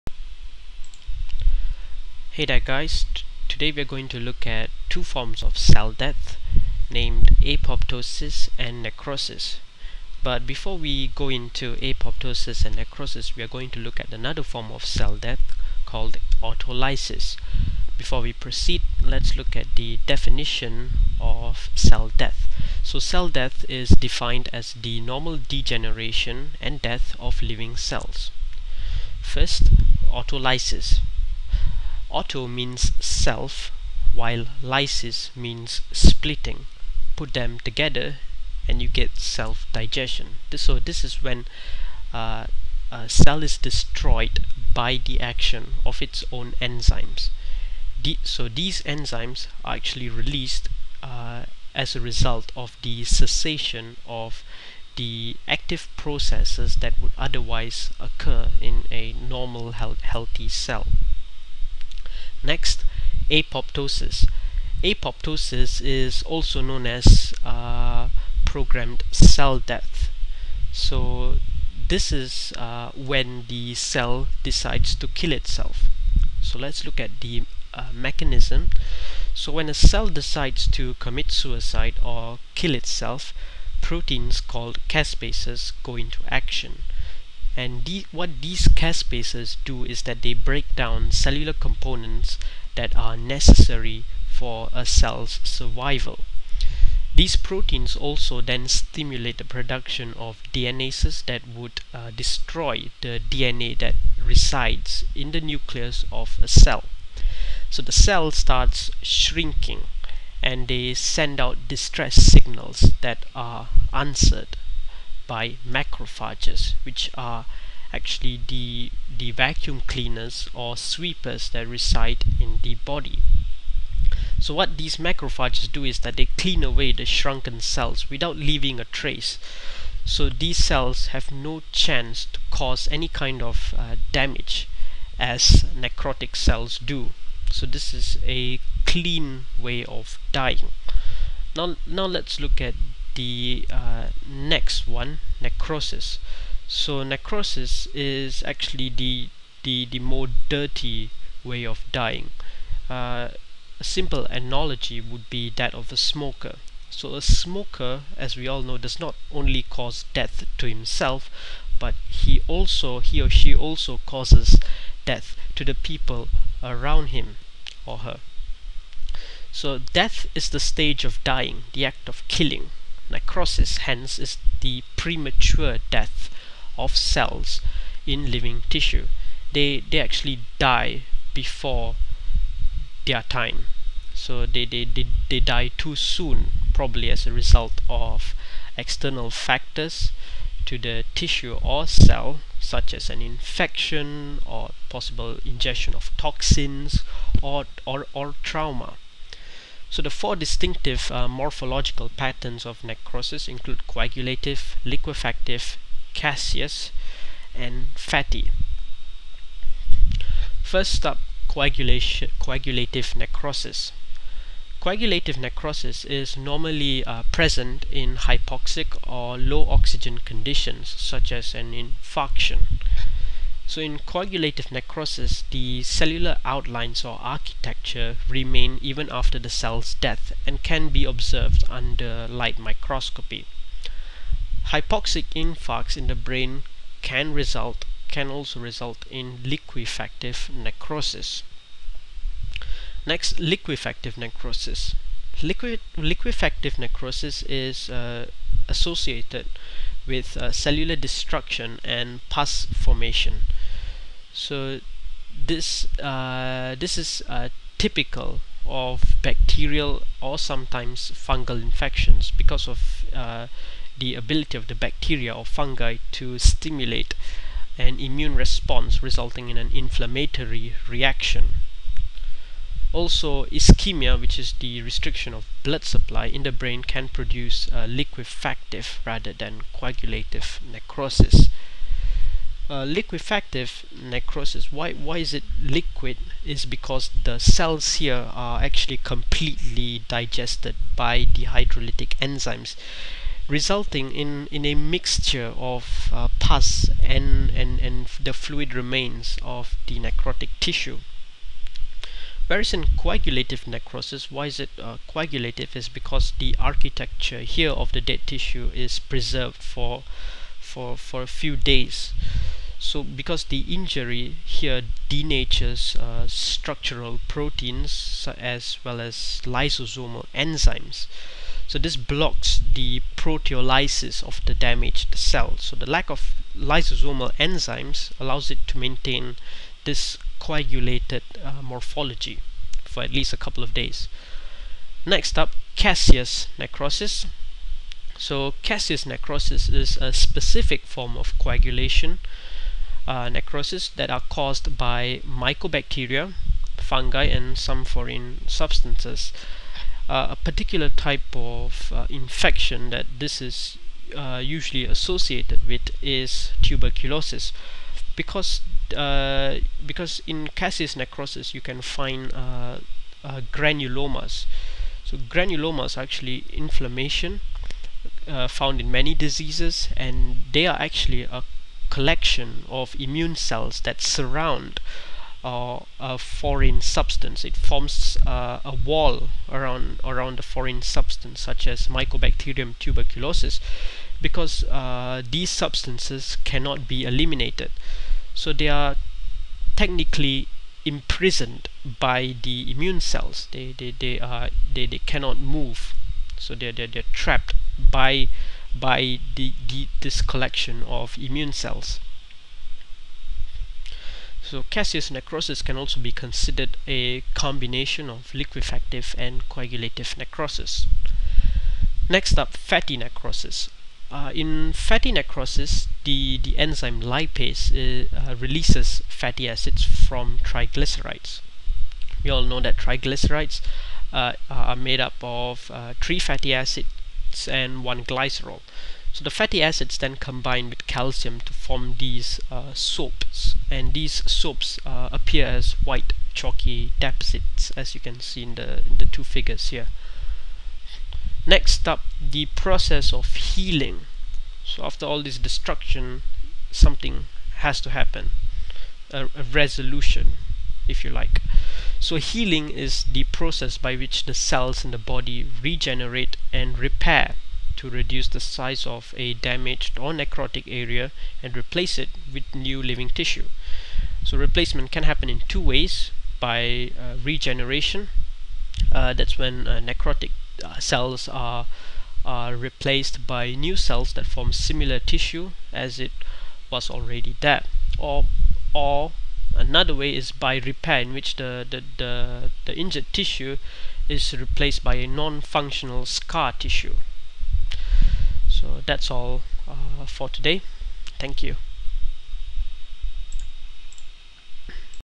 Apoptosis & Necrosis
This is a video lecture on the two forms of cell death; Apoptosis and Necrosis. Note that this video includes rudimentary explanations.